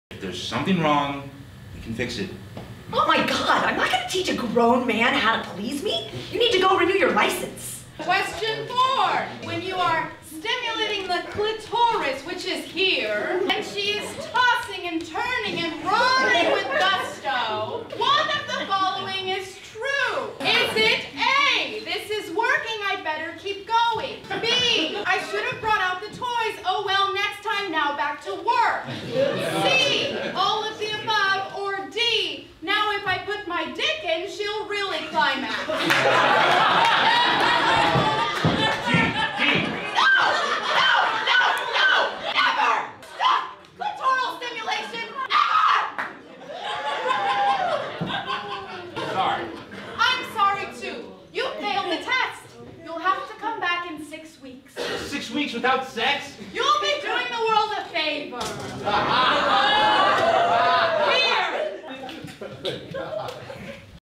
Sex Ed Test (Sketch in a minute)
Preview of Sex Ed Test
Performed at PianoFight, San Francisco
Written by Edna Mira Raia